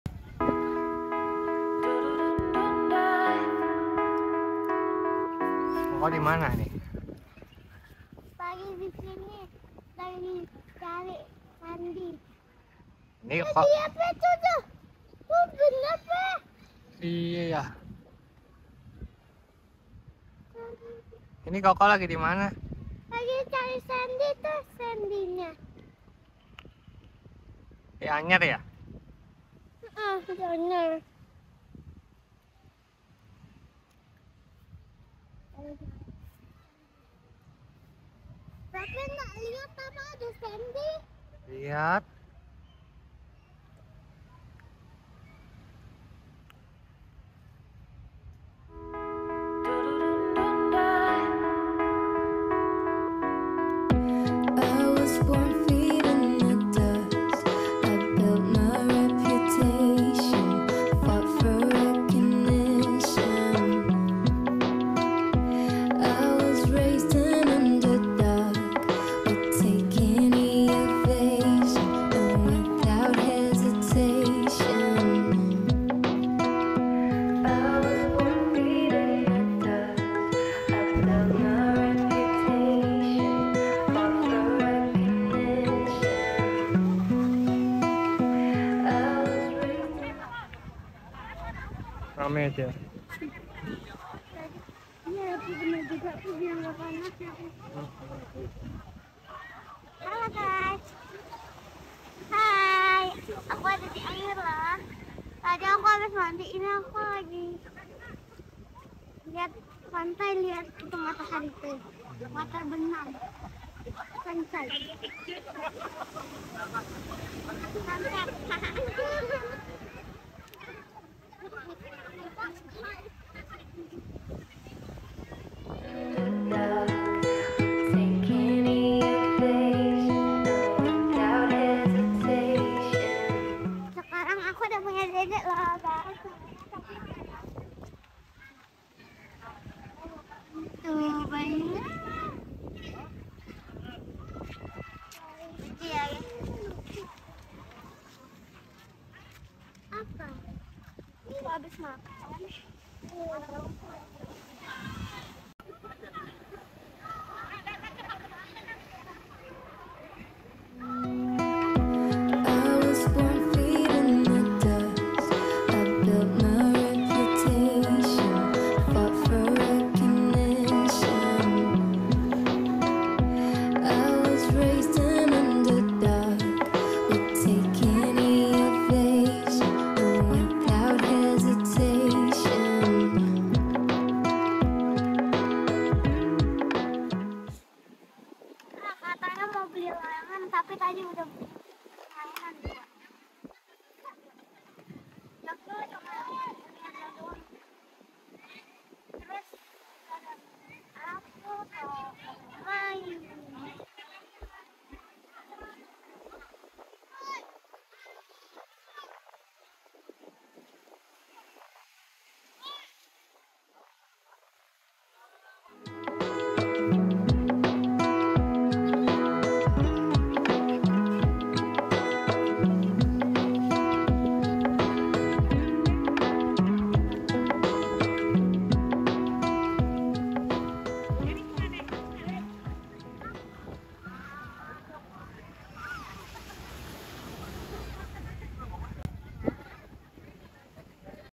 Menikmati sunset di Pantai Anyer langsung dari halaman hotel 🌊 - Priscilla HS
Priscilla HS and Family

We are love to traveling around. Video ini dokumentasi lama kami, kami sering berkunjung dan menginap disini, Anyer Cottage salah satu hotel favorit kami untuk menginap saat berkunjung ke Anyer.